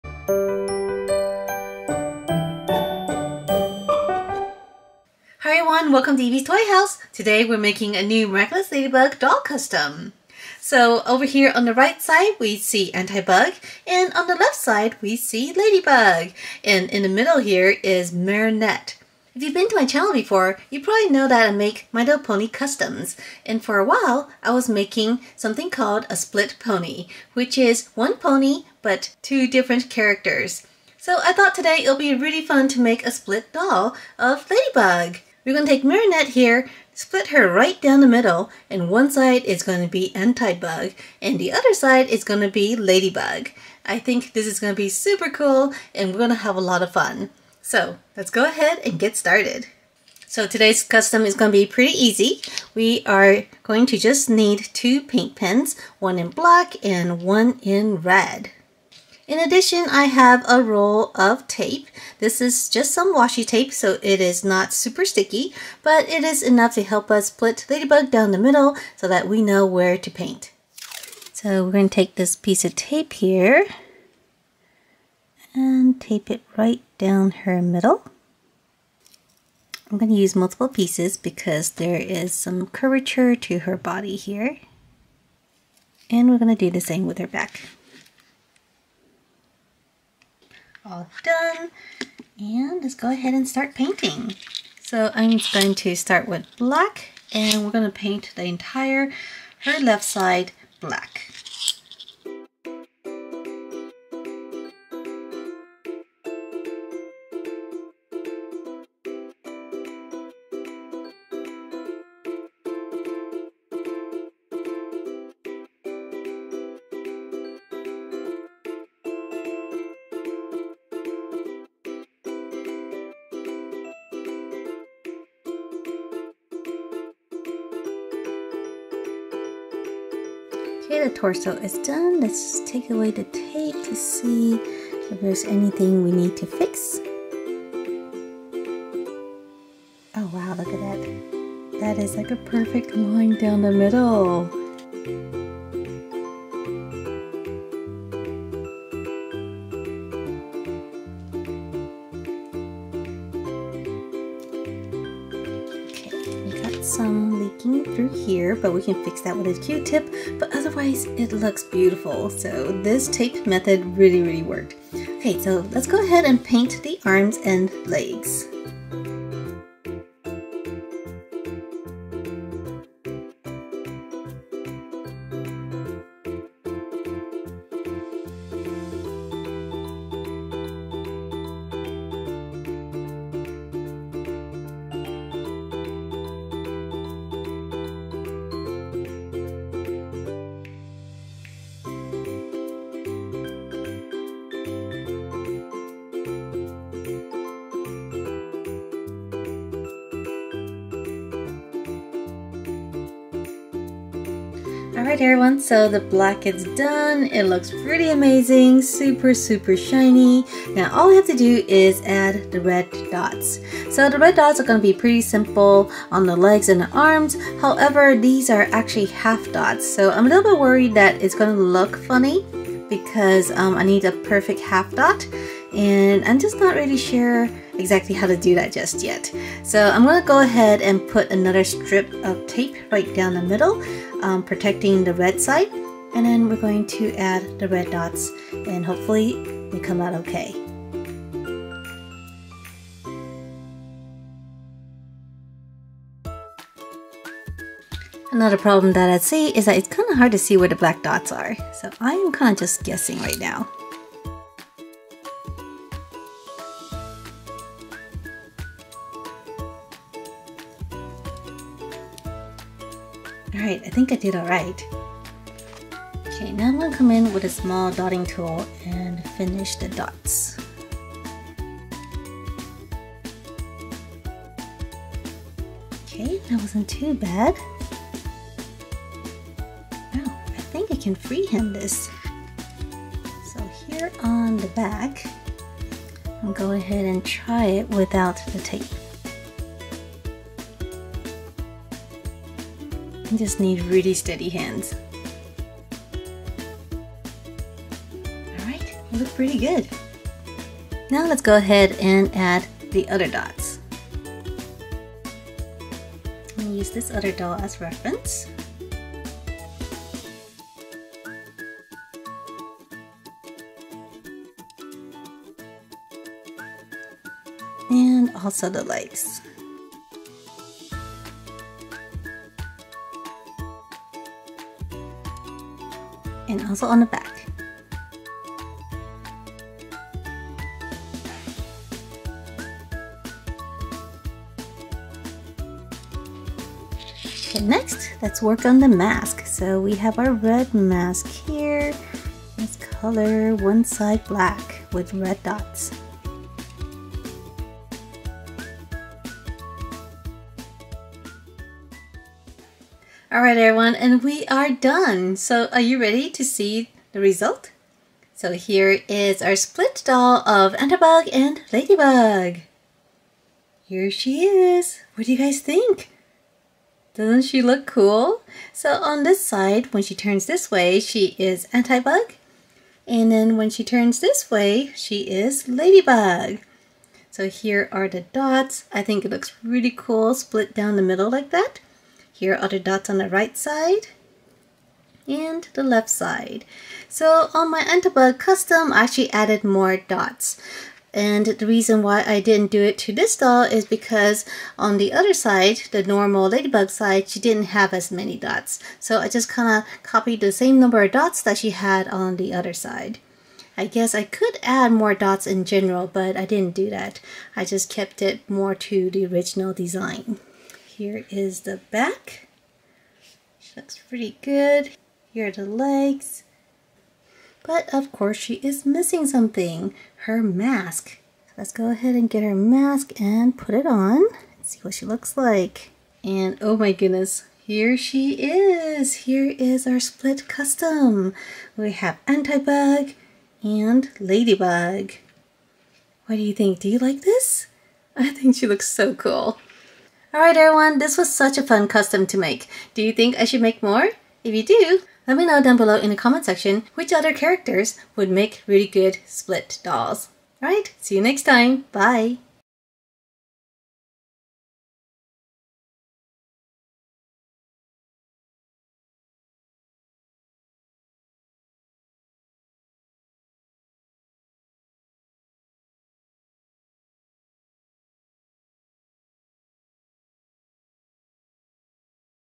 Miraculous Ladybug and Antibug Villain Custom Split Toy Doll
What happens when Ladybug gets akumatized and turns into Antibug? Let's make a custom split toy doll that shows both Ladybug and Antibug in the same doll. The two Ladybug characters will be split right down the middle.

DIY Custom Supplies